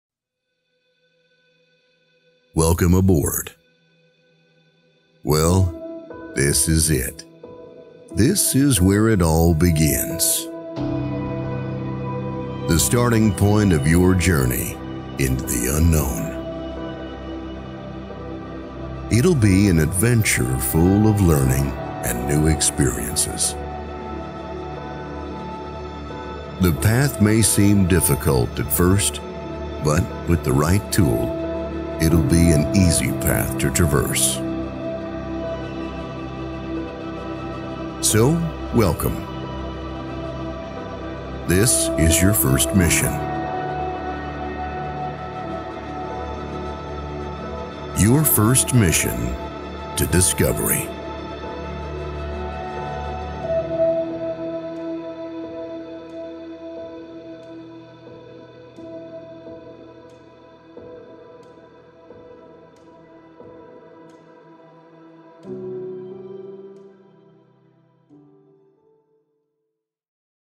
MP Cadet 3D Printer - Teaser
Ready for the First Mission to Discovery.
The MP Cadet 3D Printer is suitable for anyone looking to embark on a 3D printer journey, this printer is perfect for both beginners and experienced users!
Coming Soon!